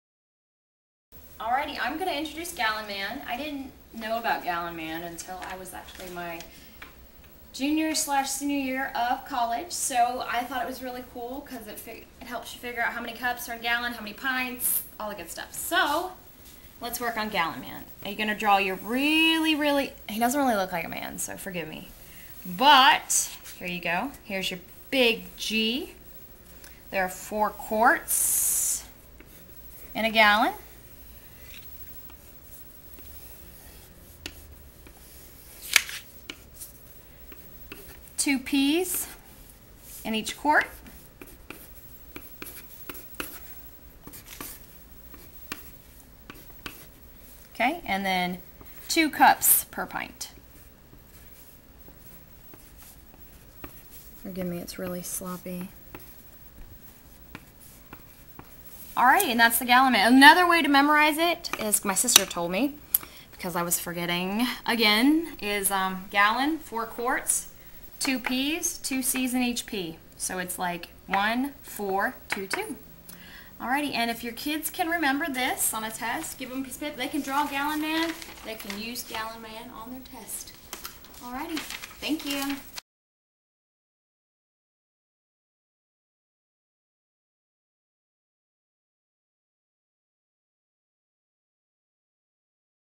Introducing Gallon Man
Okay yes he does not really look like a man. However, he is helpfull on  finding out how many quarts, pints, cups, are in a gallon.
Still today I use the gallon man!